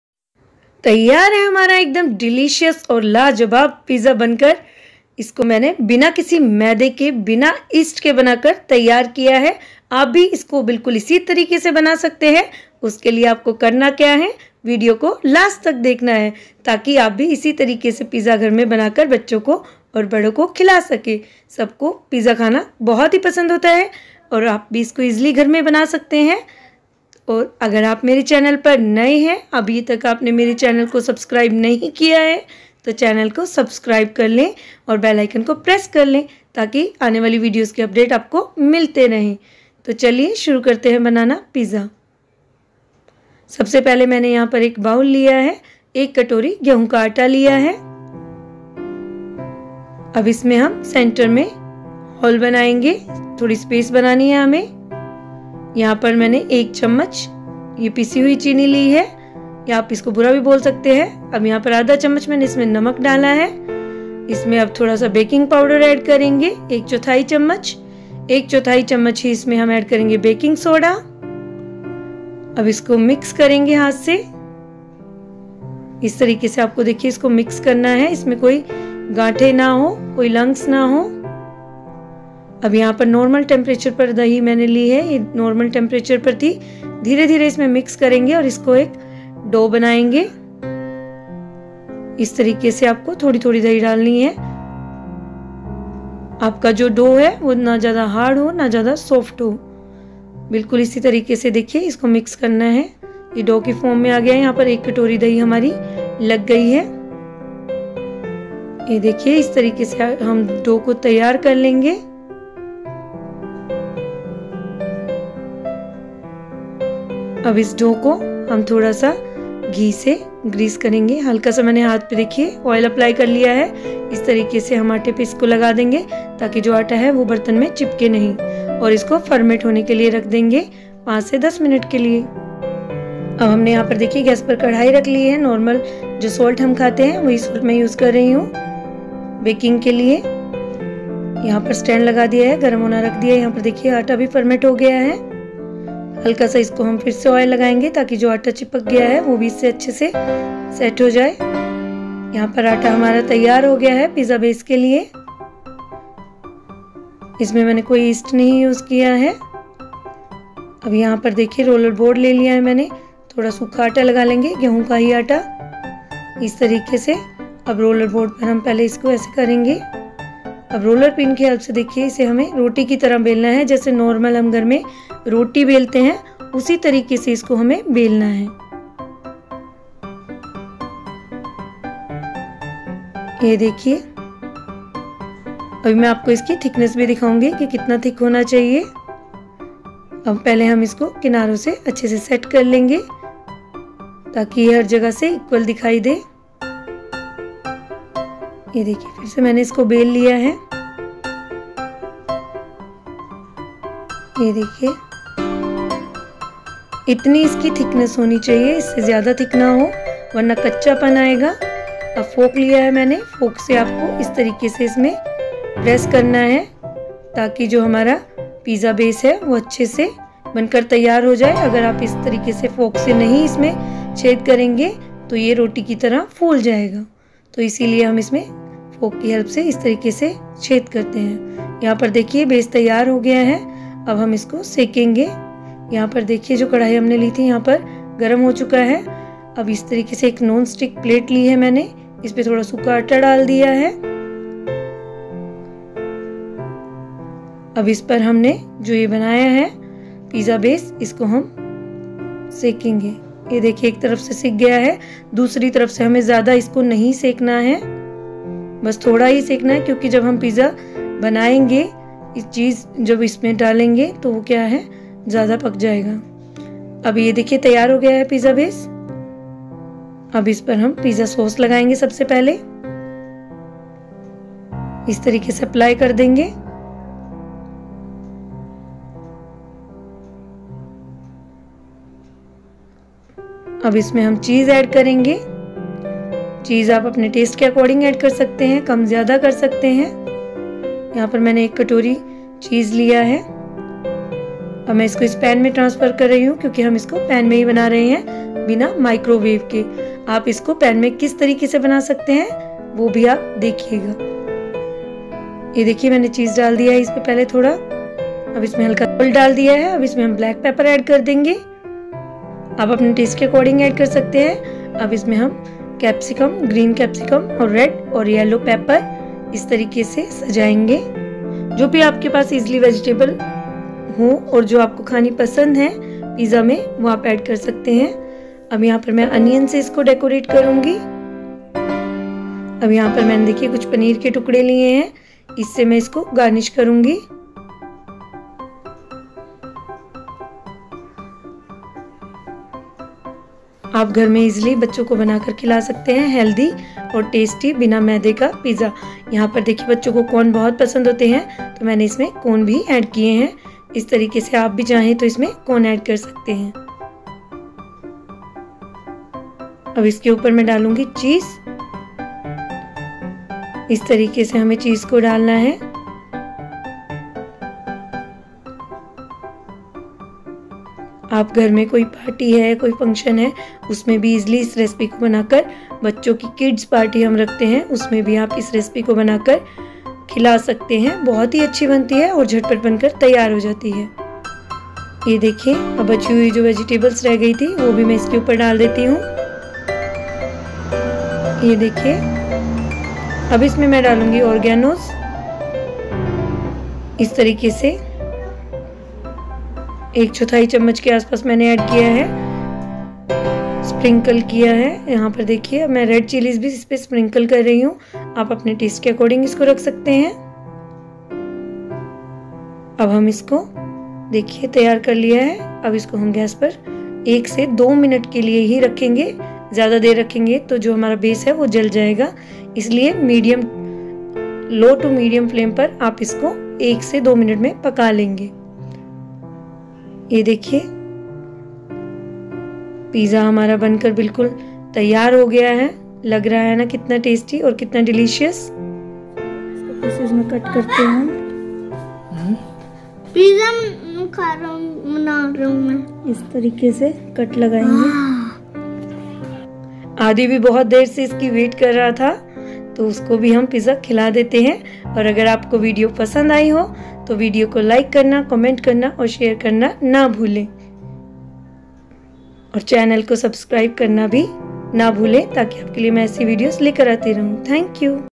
Pizza Without Maida - बिना मैदा बिना ओवन घर पे ही बनाये गेहू के आटे का पिज्जा - आज ही बनाये- Sonia
Ingredients:

For dough :

wheat flour - 1 bowl (big)

dahi (fresh)-1 bowl (small)

salt -for baking

For garnishing:

pizza sauce - 1-2 tablespoon

chesse disc - 1 bowl

vegetable - ½ piece green Capsicum, ½ piece red bell pepper ( red Capsicum) , ½ piece yellow bell pepper ( yellow Capsicum), onion-½ small size,

frozen corns - ½ small bowl

salt - pinch off (chutki bhr)

oregano (Green herbs) - as per taste

red Chile flakes - as per taste

how to make aate ka pizza at home